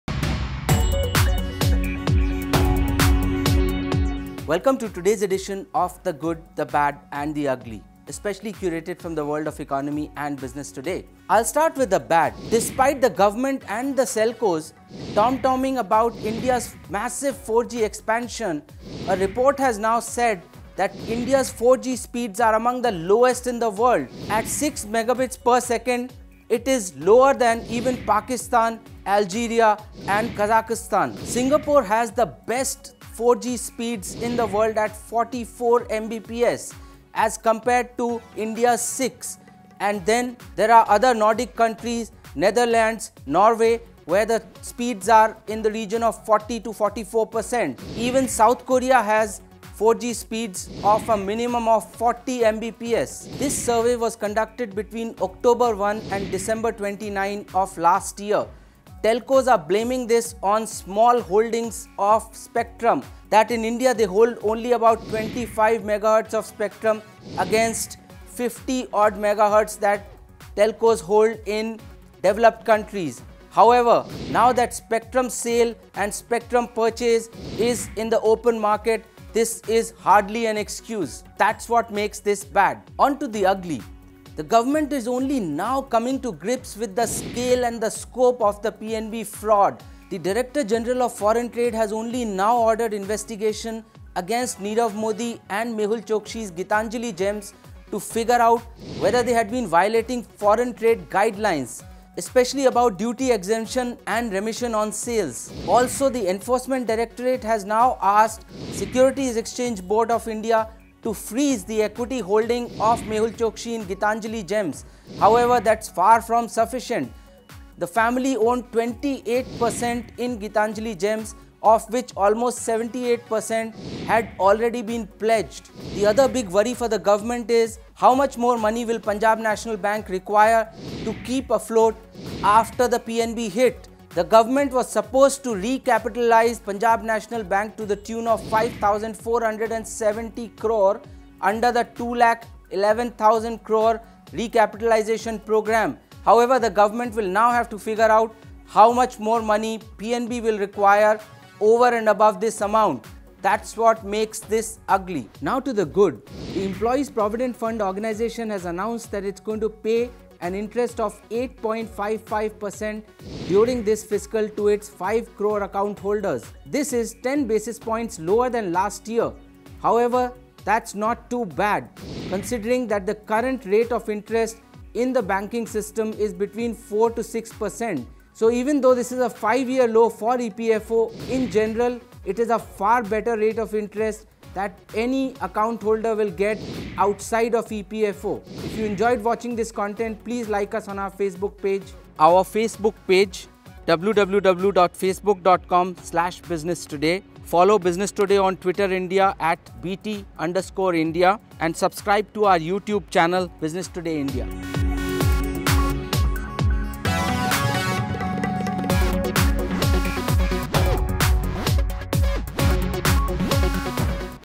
India's 4G speeds among the lowest in the world and more on The Good, the Bad and the Ugly
Despite the government and the telcos tom-toming about India's massive 4G expansion, a report has now said that India's 4G speed are among the lowest in the world at 6mbps. It is lower than even Pakistan, Algeria and Kazakhstan; The government is now coming to grips with the scale and scope of the PNB scam. The director-general of foreign trade has now only ordered an investigation against Nirav Modi and Mehul Choksi's Gitanjali Gems to find out whether they had been flouting foreign trade guidelines, especially about duty exemption and remission on sales; Employees Provident organisation has announced that it will pay an interest of 8.55% during this fiscal year to its 5 crore account holders This is 10 basis point lower than last year. BusinessToday.in Editor Rajeev Dubey elaborates in this episode of The Good, The Bad, and The Ugly.